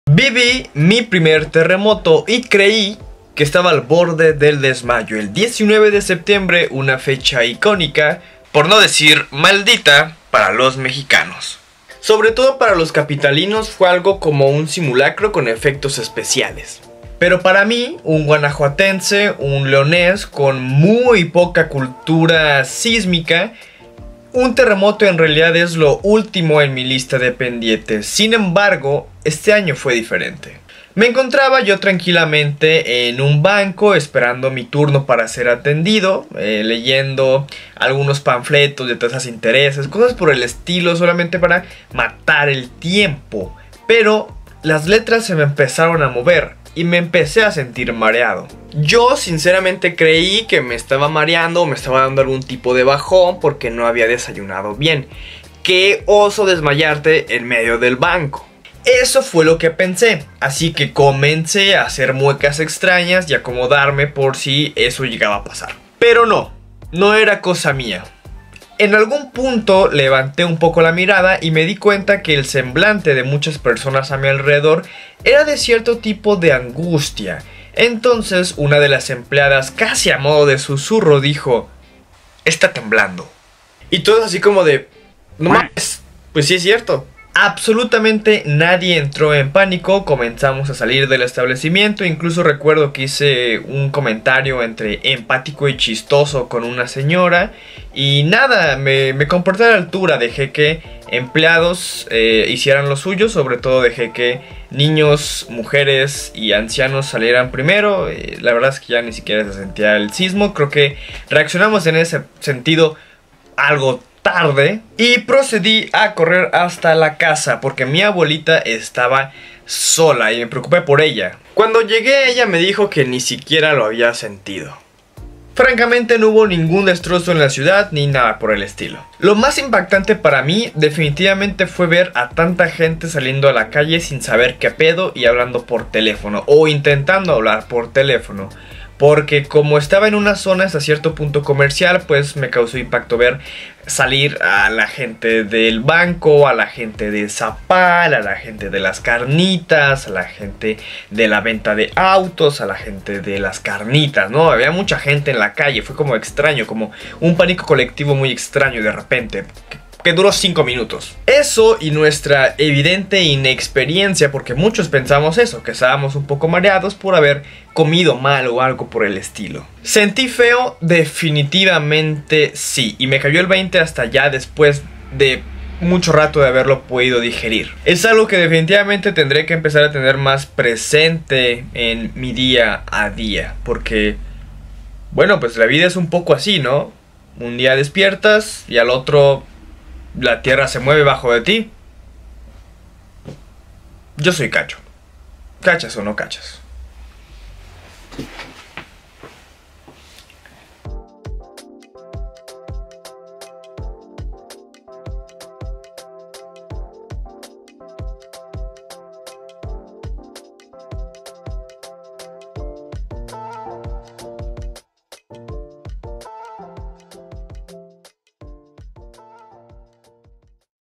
Mi Primera Vez En Un Temblor
El pasado 19 de septiembre 2022 , como ya parece ser costumbre y maldición para México , se vivio un terremoto , esta es mi experiencia en un temblor , mi primera vez en un sismo